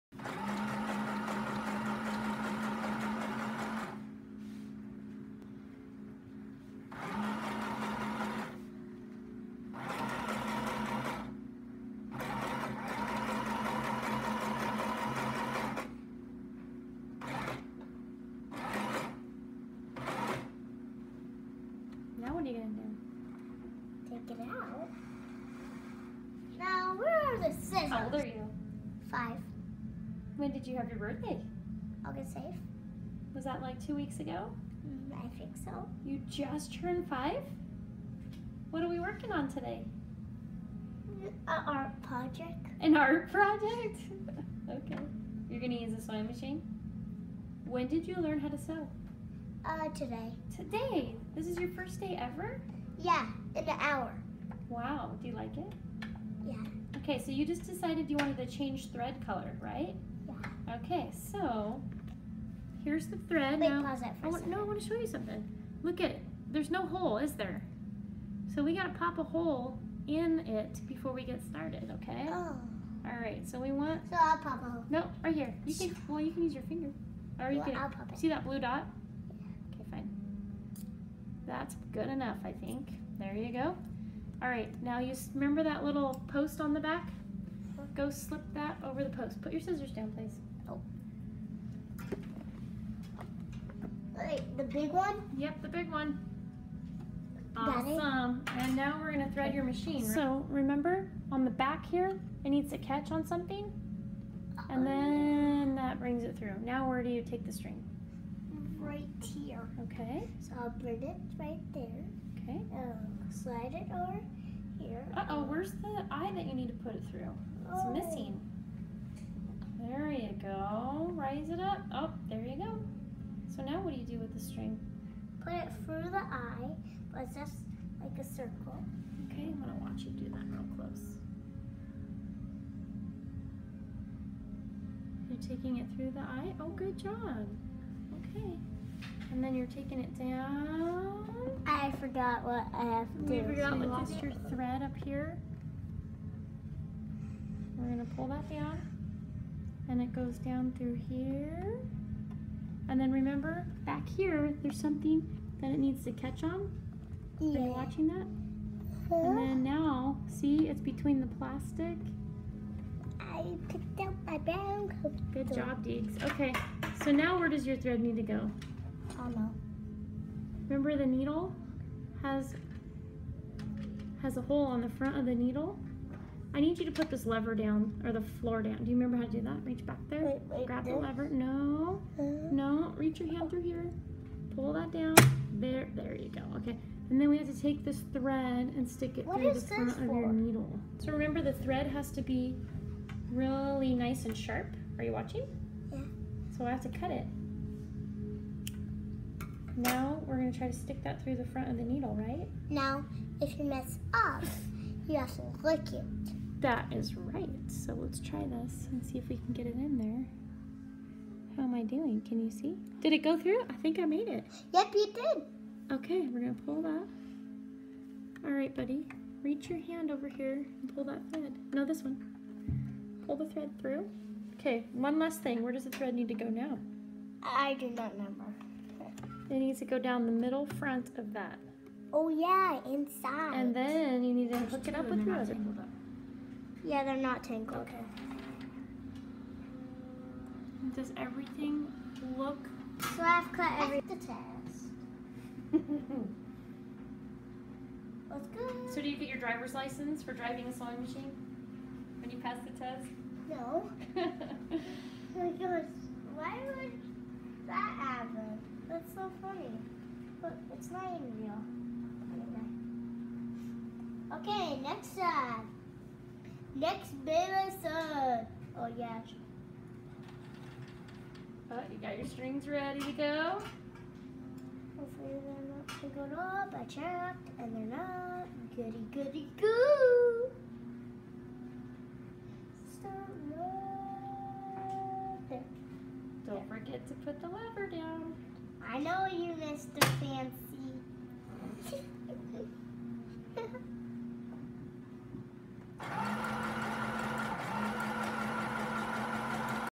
Deegan Loves ALL Machines, Even Sewing Machines
5 year old thread a Janome Sewing Machine